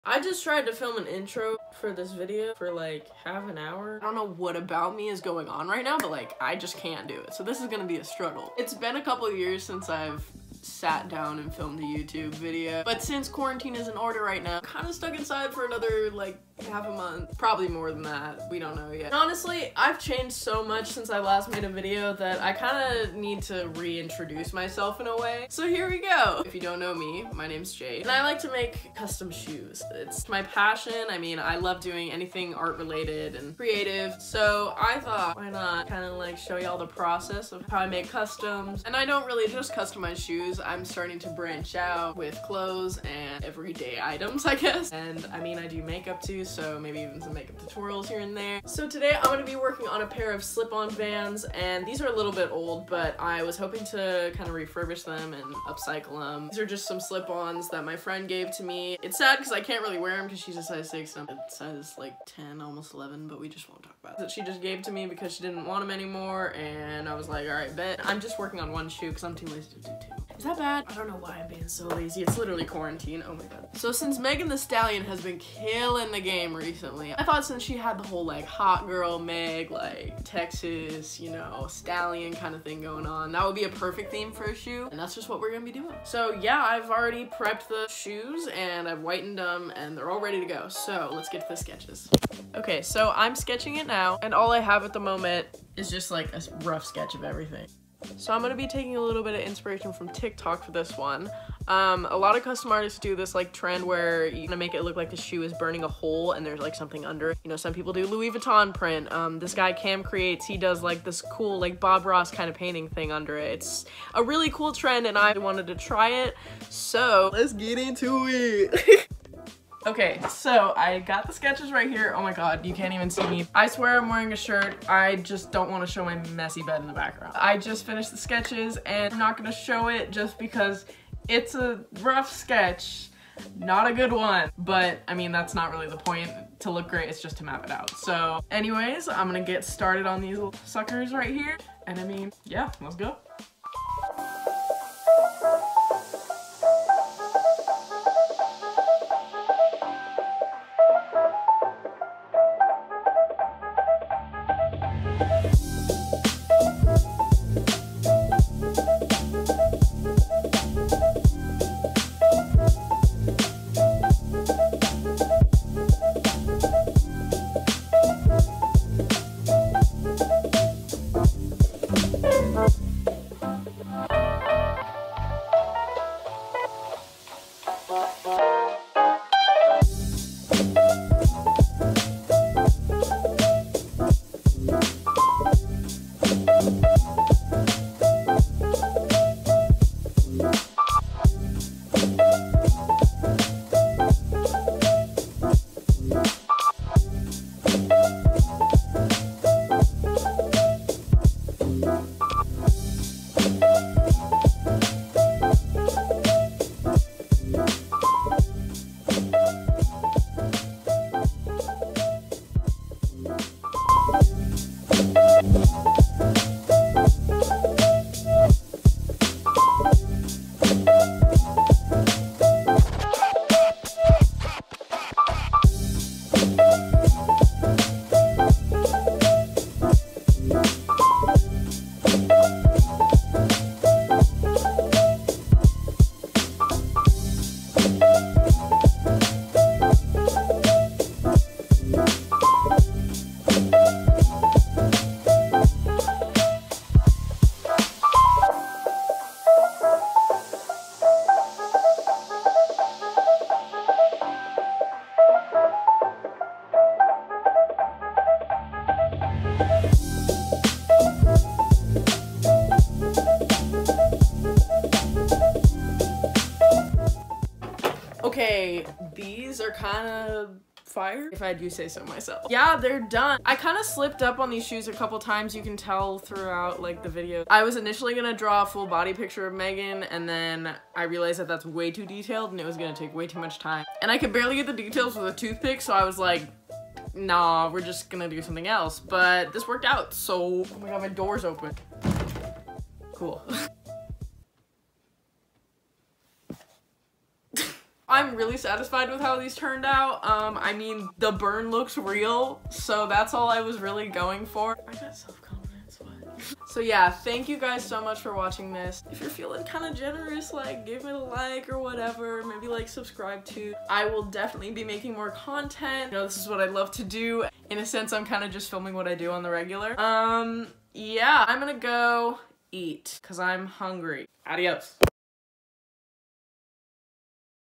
Upcycled Megan Thee Stallion Custom Vans!
Upcycled some old dirty vans into these Megan the stallion customs!

SNAP- @jadelawlessyo03